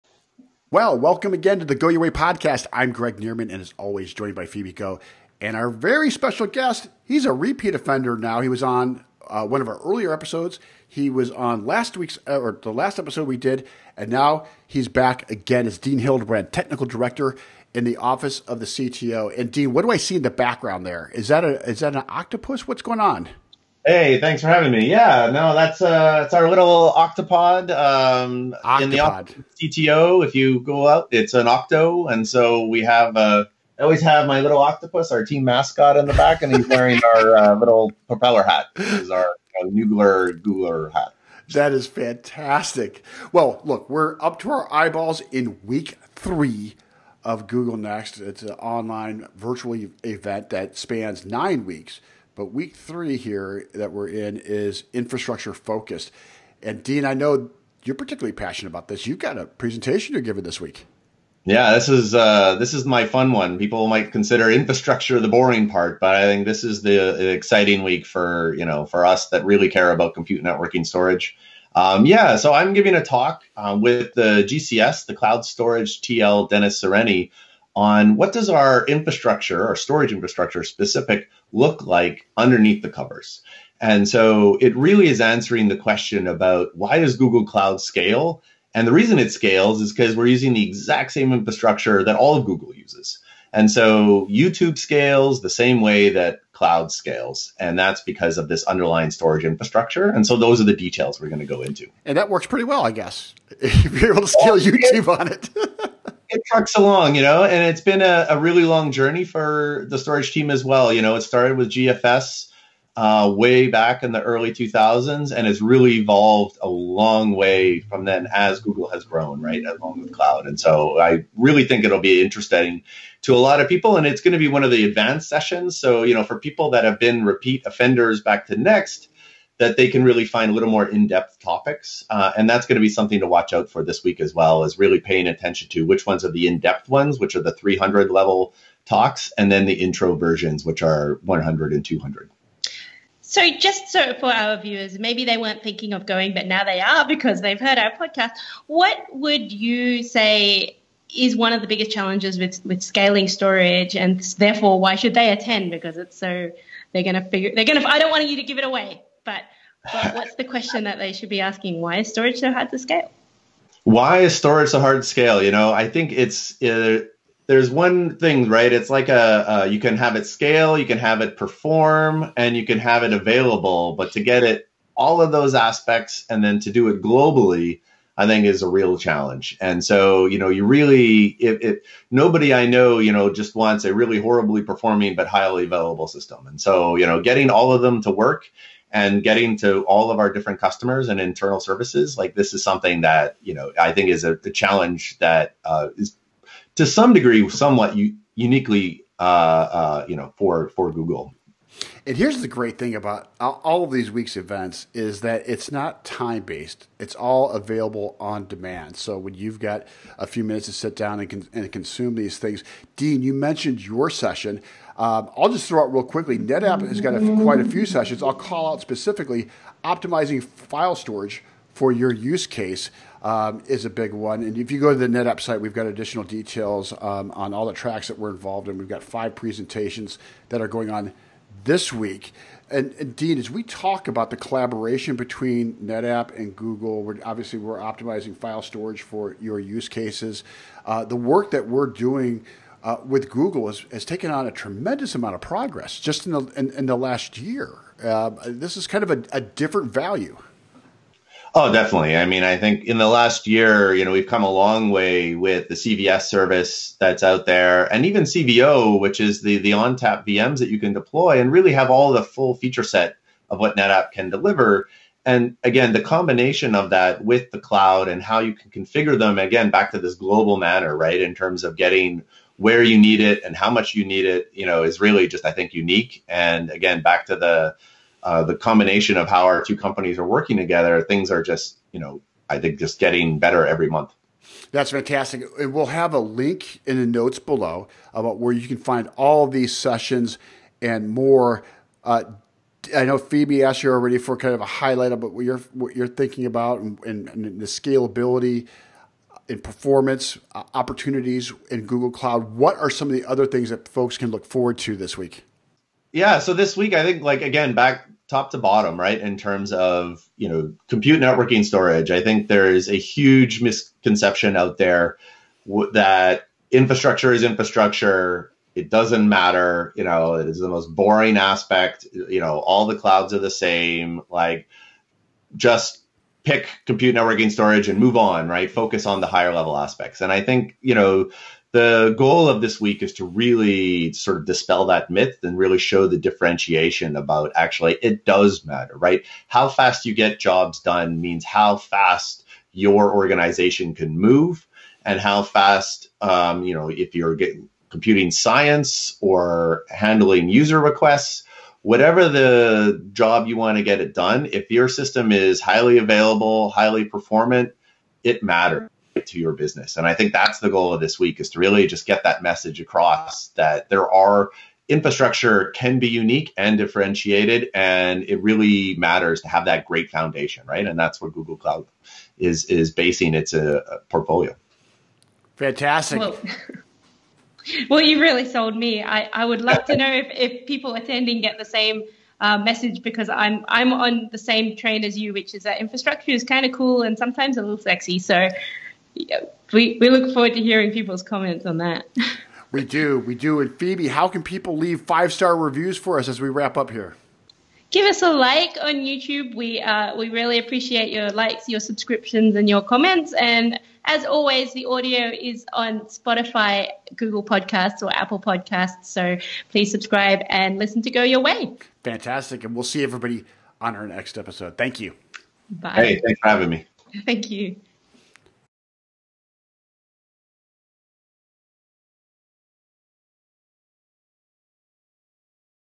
Google Cloud & NetApp -- Go Your Way Podcast
We are beginning Week 3 of Google Cloud Next 2020 with Dean Hildebrand, Technical Director in the Office of the CTO at Google. In this episode, we dive into the close work and collaboration that Google Cloud and NetApp have developed for years and how that is driving real business value for Google Cloud users. We also discuss some of the presentations and announcements from Google Cloud NEXT 2020. Want to see what else we have on offer over the next few weeks